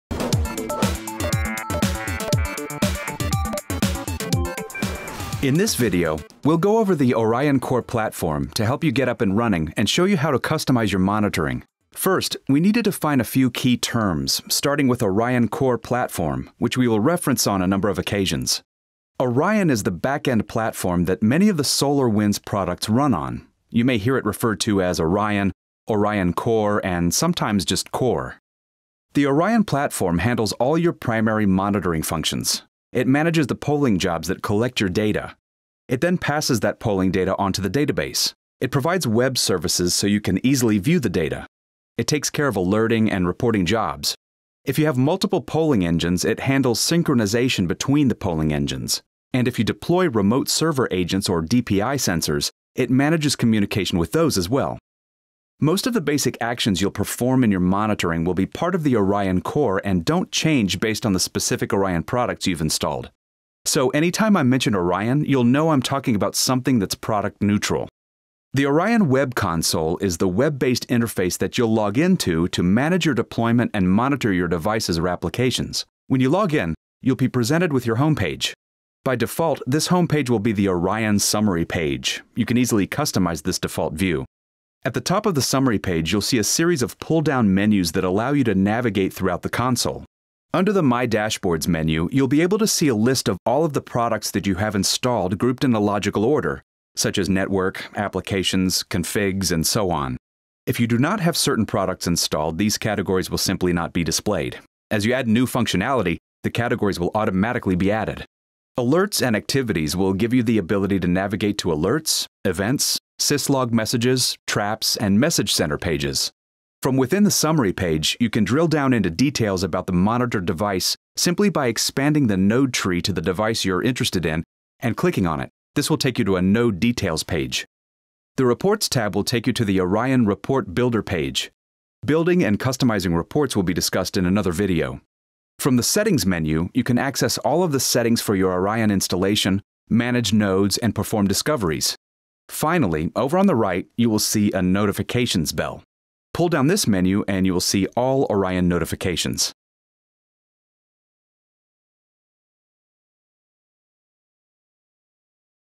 SolarWinds Network Performance Monitor Training: Navigating the Web Console
This is an overview of the Orion® platform, and how to navigate the Web Console to view alerts and troubleshoot performance issues.

The Orion platform is a set of common services for Orion-based SolarWinds® products to include Network Performance Monitor, Server & Application Monitor, Network Configuration Manager, Storage Resource Monitor, and more. The Orion platform handles all your primary monitoring functions. It manages the polling jobs that collect your network data and passes that polling data on to a common database. It provides web services so you can easily view the data and it takes care of alerting and reporting jobs. If you have multiple polling engines, it handles synchronization between the polling engines. If you deploy remote server agents or DPI sensors, it manages communication with those as well.